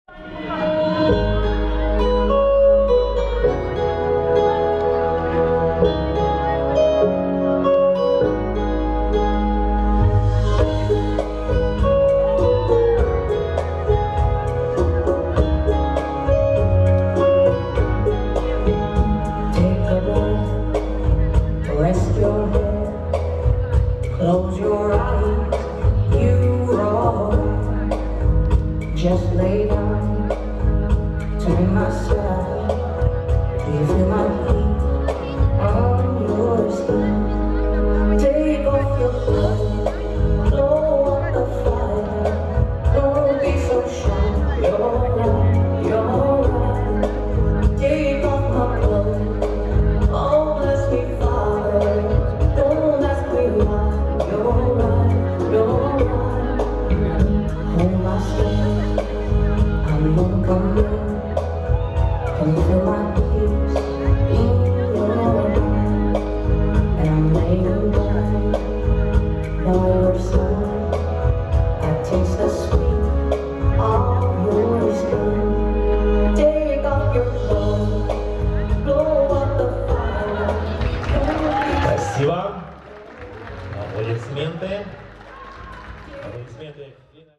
Пашкович Макар и Асадчая Ксения Rumba🏆 AllianceTrophy2023🏆
Международные соревнования по танцевальному спорту
10-летие Белорусского альянса танцевального спорта
Организатор: Ассоциация "БАТС"

Couple №33: Пашкович Макар и Асадчая Ксения
(Club: Танго, Minsk, Belarus)
Trainer (s): Ярцев Павел Петрович  и Баслык Марина Ростиславовна